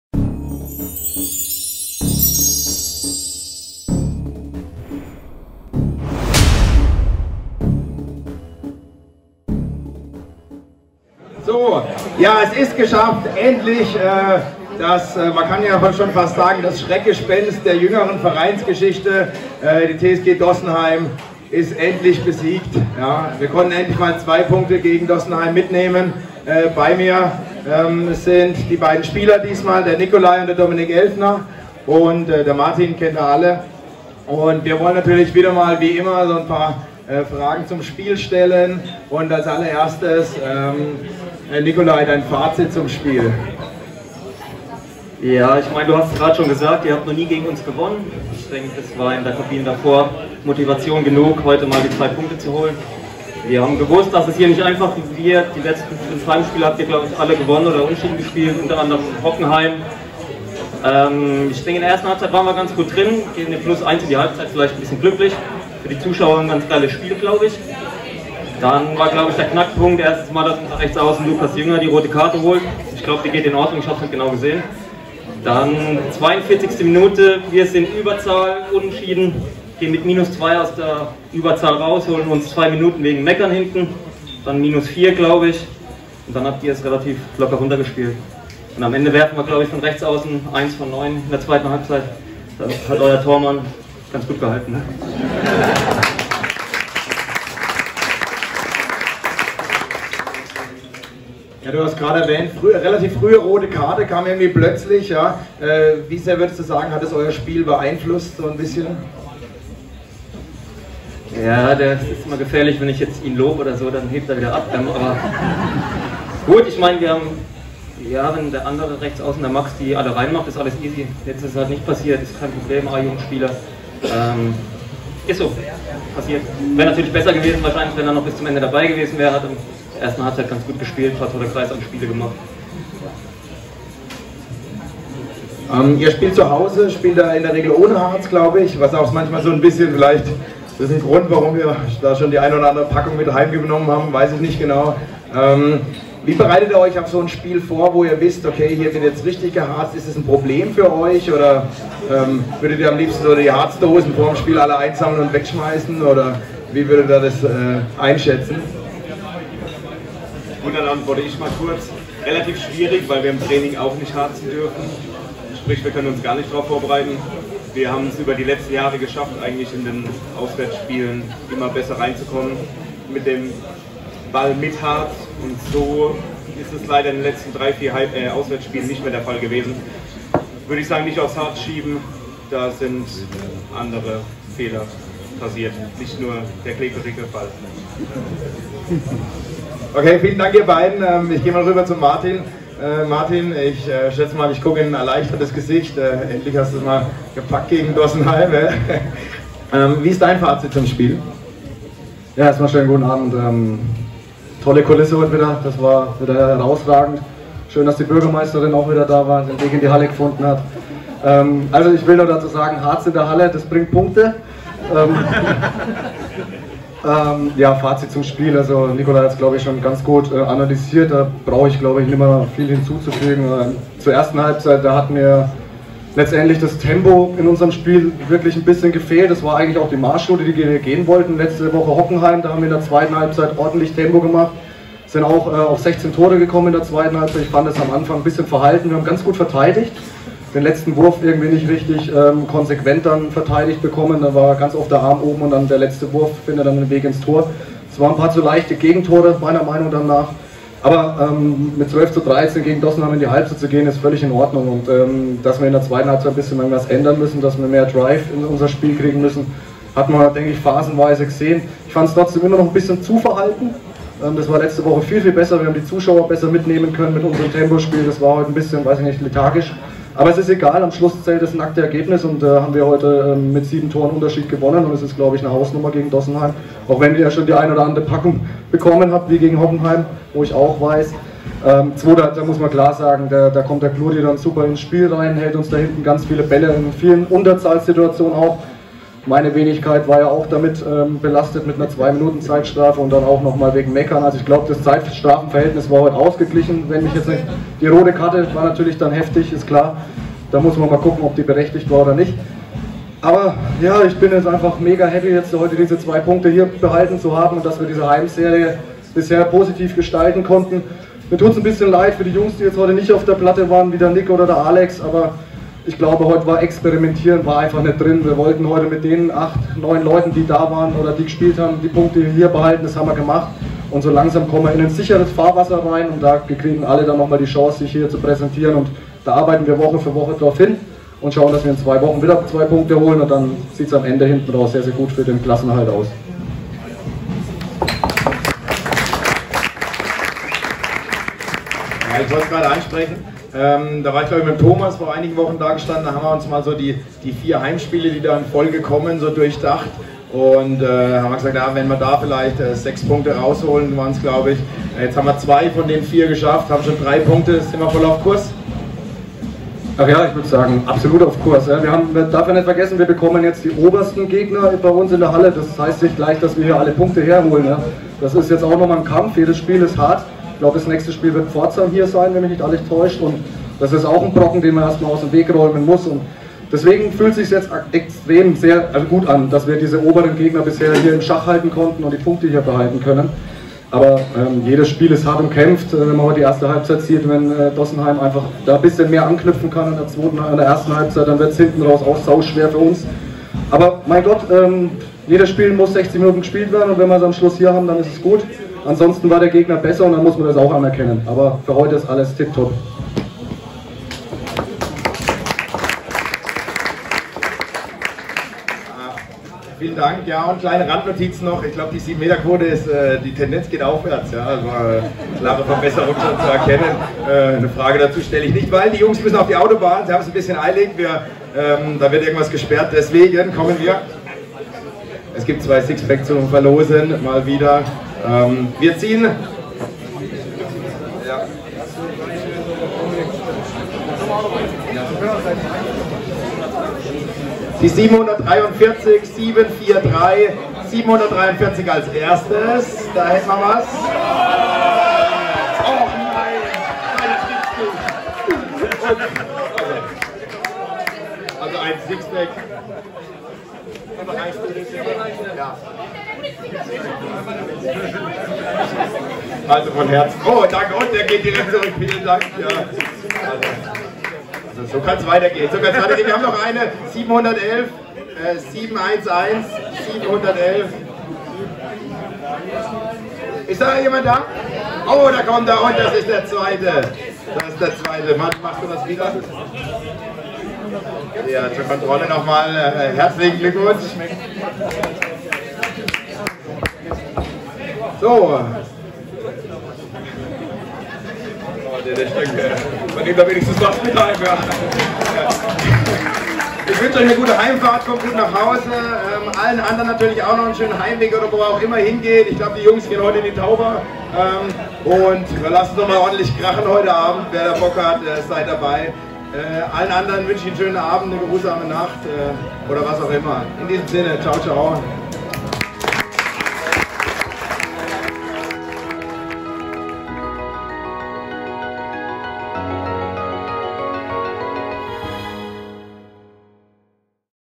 TAUBERTALK am 15.02.2020 - Heimspiel gegen die TSG Germania Dossenheim
Es war ingesamt wahrlich kein Handball-Leckerbissen, aber eine kämpferisch starke Mannschaftsleistung der HSG Dittigheim/Tauberbischofsheim gegen die TSG Germania Dossenheim "Die Erste" - Endstand 27:20!

Hier die Stimmen zum Spiel von dem Gästetrainer Nicolai Elfner und unserem Chefcoach Martin Keupp